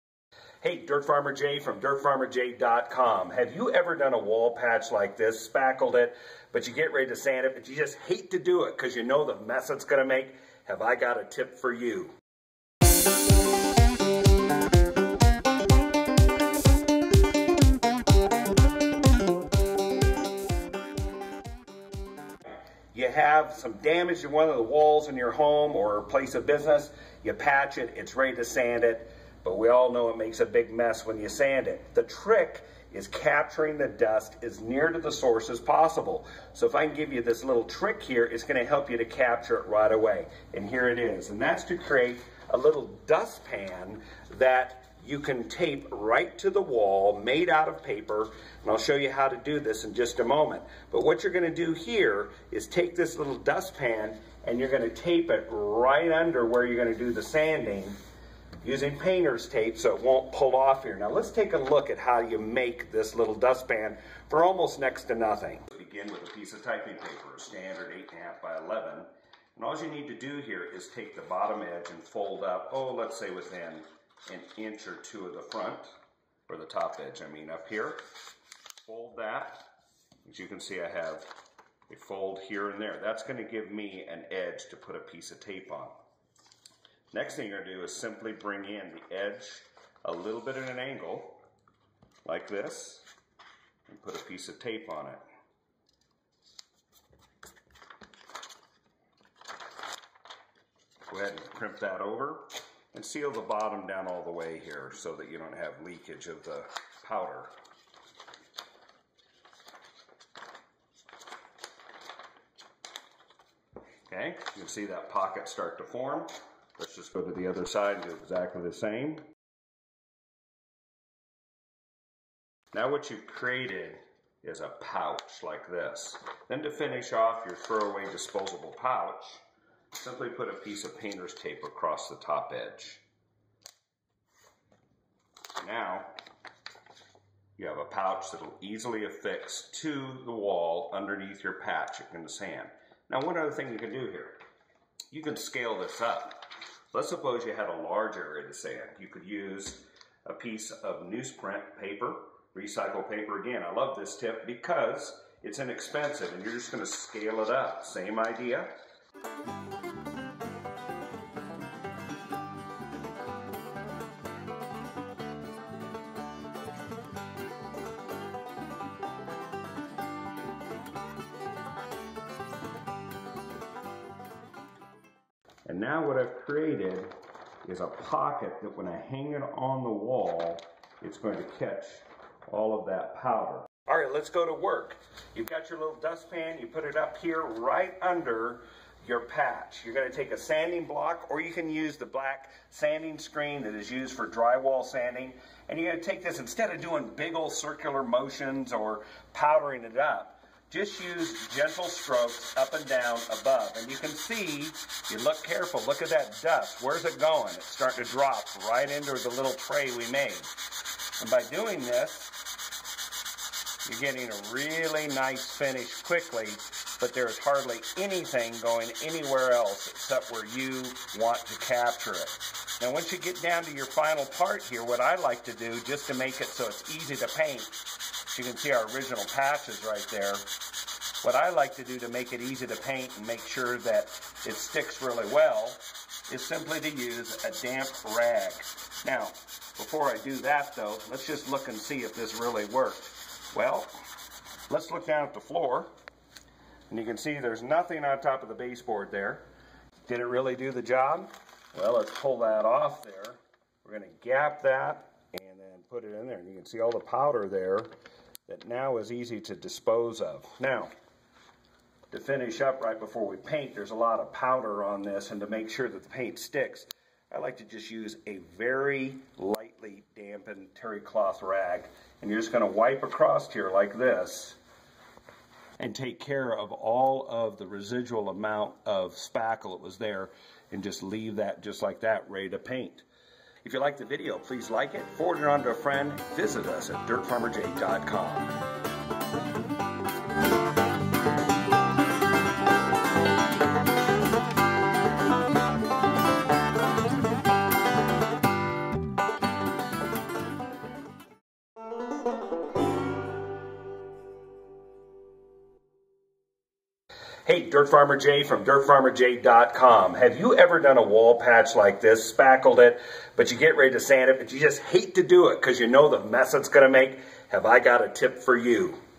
Small Drywall Patches without the Mess!
Do you avoid repairing gashes or holes in your walls because it's such a hassle and makes a mess? Dirt Farmer Jay has a great tip that will ease your pain.